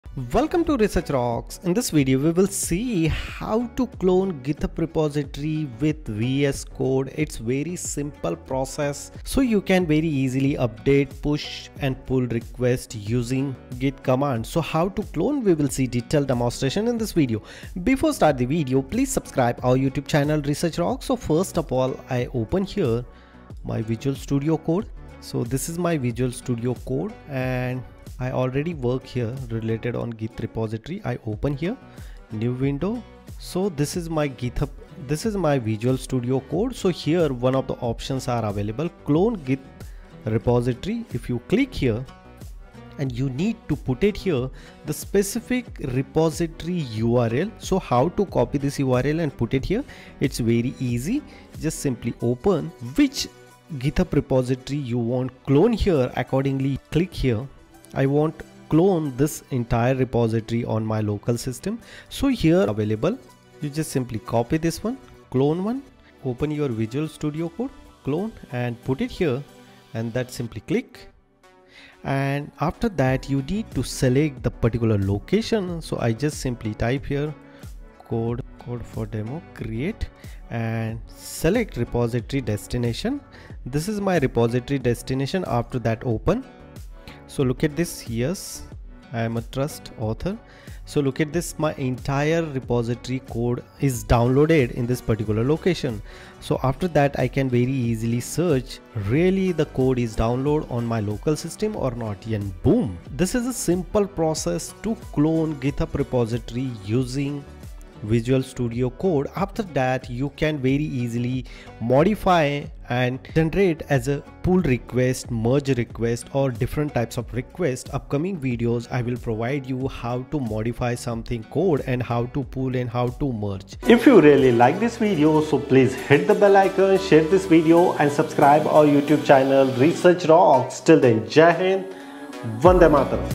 How to Clone GitHub repository with Visual Studio Code | Clone GitHub repository in VS Code
We'll see in this video, You can search for and clone a repository from GitHub using the Git: Clone command in the Command Palette (Ctrl+Shift+P) or by using the Clone Repository button in the Source Control view (available when you have no folder open).